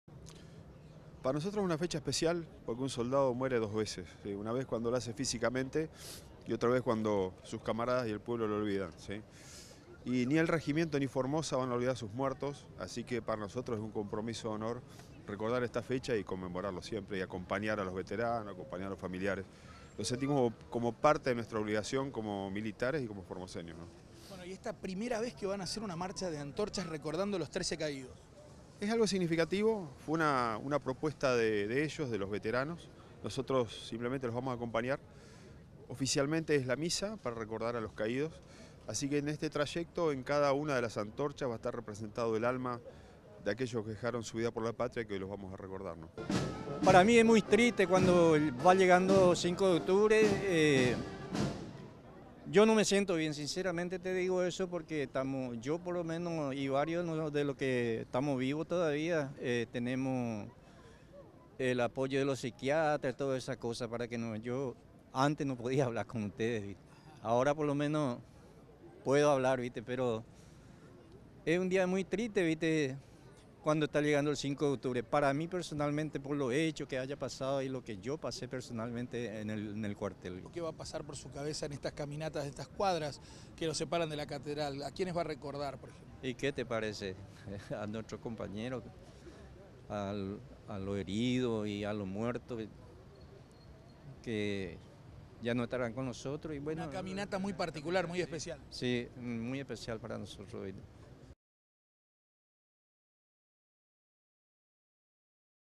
MARCHA DE ANTORCHAS Y MISA POR LOS CAÍDOS DEL 5 DE OCTUBRE
SE CUMPLEN 47 AÑOS DE LA HEROICA DEFENSA DEL REGIMIENTO DE INFANTERÍA DE MONTE 29 Y SE REALIZÓ, ANOCHE, UNA MARCHA DE ANTORCHAS Y MISA EN LA CATEDRAL EN HOMENAJE A LOS SOLDADOS CAÍDOS EN COMBATE. FUE UNA PROPUESTA DE LOS VETERANOS PARA RECORDAR A CADA UNO DE LOS 13 COMPAÑEROS FALLECIDOS.